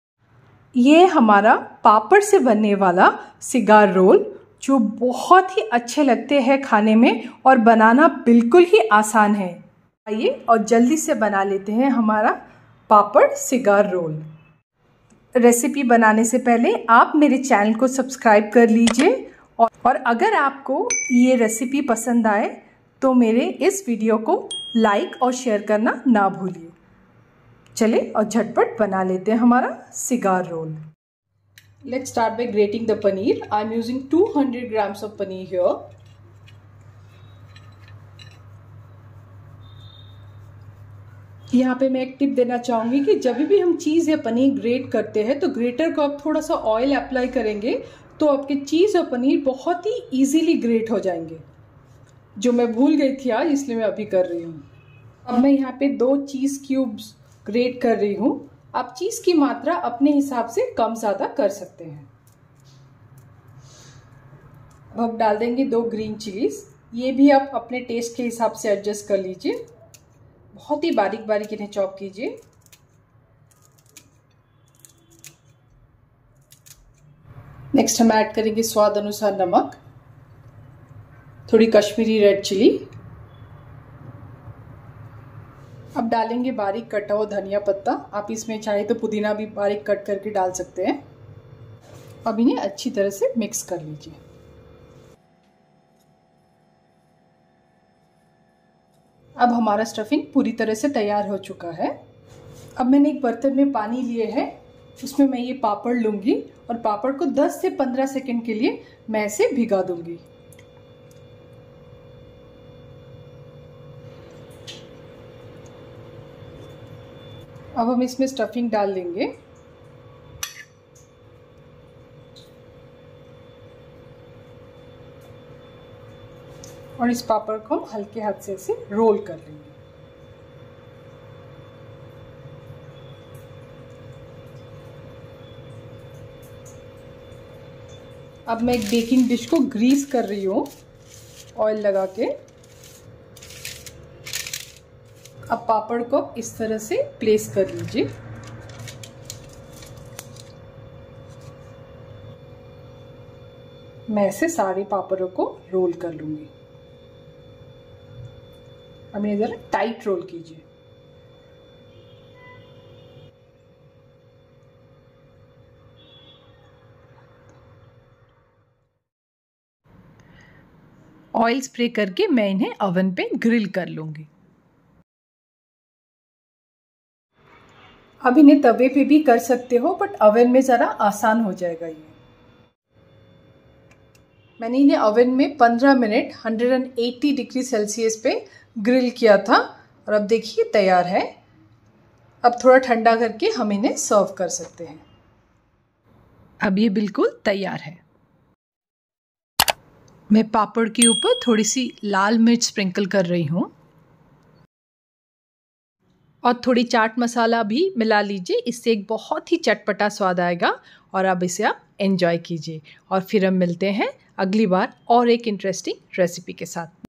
Papad Cigar Roll recipe | Best snack made from papad
Papad Cigar Roll is a delicious and crispy Indian snack made from papads, which are thin, disc-shaped lentil wafers. This recipe adds a savory filling and rolls them into cigar-like shapes. Here's a simple recipe to make Papad Cigar Rolls:

Ingredients:
6-8 medium-sized papads (you can use any flavor you like, like plain, masala, or urad dal papads)
Paneer Cube - 2
1-2 green chili, finely chopped (adjust to your spice preference)
1/2 teaspoon red chili powder (adjust to your spice preference)
1/2 teaspoon chaat masala
1 tablespoon chopped coriander leaves

Instructions:
In a mixing bowl, combine grated Paneer, grated Cheese , green chili,  red chili powder, chaat masala, chopped coriander leaves, and salt. Mix well to form a smooth filling. Adjust spices according to your taste.

Take a papad and slightly dampen it with water on both sides.

Place a tablespoon of the filling mixture on one end of the papad and shape it into a thin, long cylindrical roll.

Carefully roll the papad from the filling end, making sure to roll it tightly like a cigar.

Seal the edges of the papad roll by pressing gently. Repeat the process for all the papads.

Grill the Papad Rolls in Oven for 15 min at 180 degree Celsius
Serve the papad cigar rolls hot with chutney, ketchup, or any dip of your choice.

Enjoy these crunchy and delicious Papad Cigar Rolls as an appetizer or snack for parties, gatherings, or even as a teatime treat!